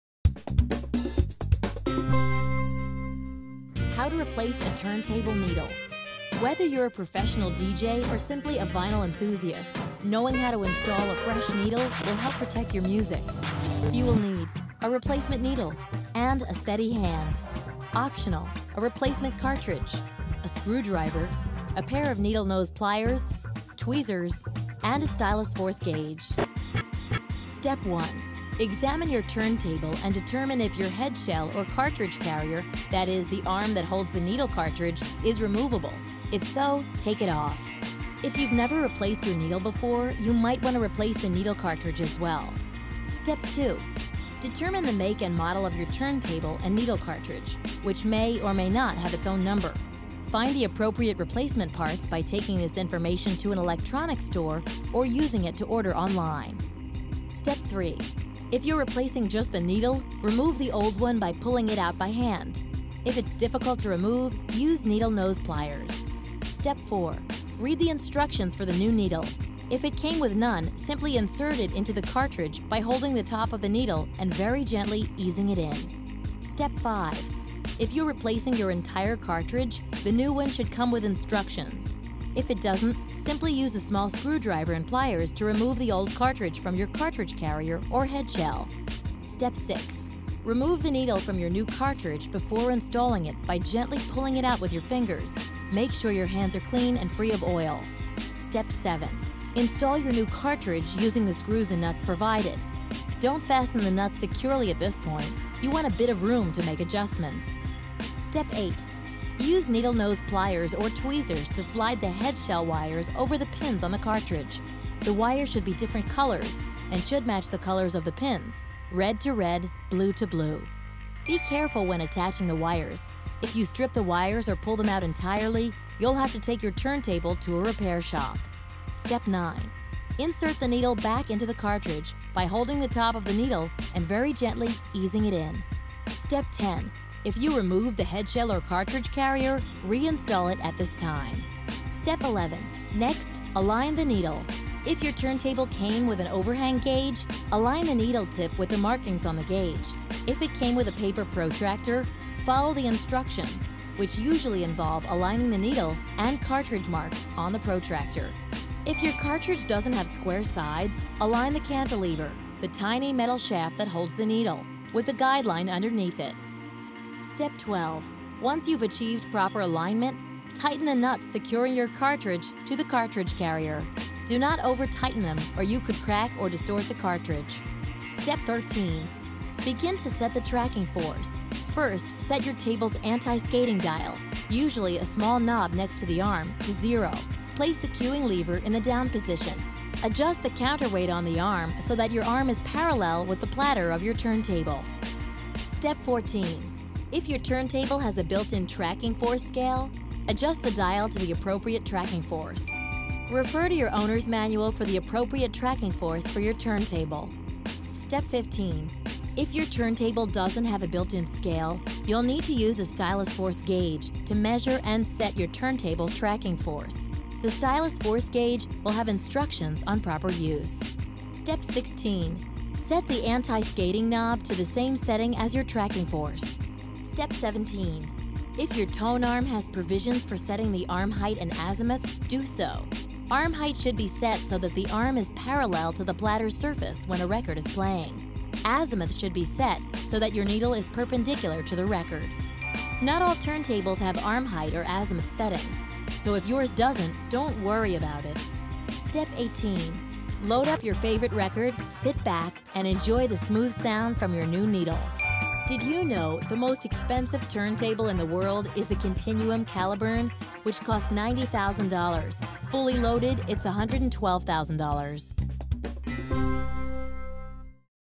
How to Replace a Turntable Needle
Whether you’re a professional DJ or simply a vinyl enthusiast, knowing how to install a fresh needle will help protect your music.

Step 1: Remove the shell or carrier
Examine your turntable and determine if your head shell or cartridge carrier—that is, the arm that holds the needle cartridge—is removable. If so, take it off.

Tip
If you’ve never replaced your needle before, you might want to replace the needle cartridge as well.

Step 2: Get make and model
Determine the make and model of your turntable and needle cartridge (which may or may not have its own number). Find the appropriate replacement parts by taking this information to an electronics store or using it to order online.

Step 3: Remove old needle
If you’re replacing just the needle, remove the old one by pulling it out by hand. If it’s difficult to remove, use needle-nose pliers.

Step 4: Insert new needle
Read the instructions for the new needle. If it came with none, simply insert it into the cartridge by holding the top of the needle and very gently easing it in.

Step 5: Remove old cartridge
If you’re replacing your entire cartridge, the new one should come with instructions. If it doesn’t, simply use a small screwdriver and pliers to remove the old cartridge from your cartridge carrier or head shell.

Step 6: Remove new needle
Remove the needle from your new cartridge before installing it by gently pulling it out with your fingers. Make sure your hands are clean and free of oil.

Step 7: Install cartridge
Install your new cartridge using the screws and nuts provided. Don’t fasten the nuts securely at this point—you want a bit of room to make adjustments.

Step 8: Rewire
Use needle-nose pliers or tweezers to slide the head shell wires over the pins on the cartridge. The wires should be different colors, and should match the colors of the pins—red to red, blue to blue.

Tip
Be careful when attaching the wires! If you strip the wires, or pull them out entirely, you’ll have to take your turntable to a repair shop!

Step 9: Insert new needle
Insert the needle back into the cartridge by holding the top of the needle and very gently easing it in.

Step 10: Reinstall shell or carrier
If you removed the head shell or cartridge carrier, reinstall it at this time.

Step 11: Align the neddle
Next, align the needle. If your turntable came with an overhang gauge, align the needle tip with the markings on the gauge. If it came with a paper protractor, follow the instructions, which usually involve aligning the needle and cartridge marks on the protractor.

Tip
If your cartridge doesn’t have square sides, align the cantilever--the tiny metal shaft that holds the needle--with the guideline underneath it.

Step 12: Tighten nuts
Once you’ve achieved proper alignment, tighten the nuts securing your cartridge to the cartridge carrier. Do not over-tighten them, or you could crack or distort the cartridge.

Step 13: Set tracking force
Begin to set the tracking force. First, set your table’s anti-skating dial--usually a small knob next to the arm--to zero. Place the cueing lever in the down position. Adjust the counterweight on the arm so that your arm is parallel with the platter of your turntable.

Step 14: Adjust the dial
If your turntable has a built-in tracking force scale, adjust the dial to the appropriate tracking force.

Tip
Refer to your owner’s manual for the appropriate tracking force for your turntable.

Step 15: Use a gauge
If your turntable doesn’t have a built-in scale, you’ll need to use a stylus force gauge to measure and set your turntable’s tracking force. The stylus force gauge will have instructions on proper use.

Step 16: Set knob
Set the anti-skating knob to the same setting as your tracking force.

Step 17: Set tone arm
If your tone arm has provisions for setting the arm height and azimuth, do so. Arm height should be set so that the arm is parallel to the platter’s surface when a record is playing. Azimuth should be set so that your needle is perpendicular to the record.

Tip
Not all turntables have arm height or azimuth settings, so if yours doesn’t, don’t worry about it.

Step 18: Enjoy the smooth sounds
Load up your favorite record, sit back, and enjoy the smooth sound from your new needle!

The most expensive turntable in the world is the Continuum Caliburn, which costs $90,000--fully loaded, it’s $112,000!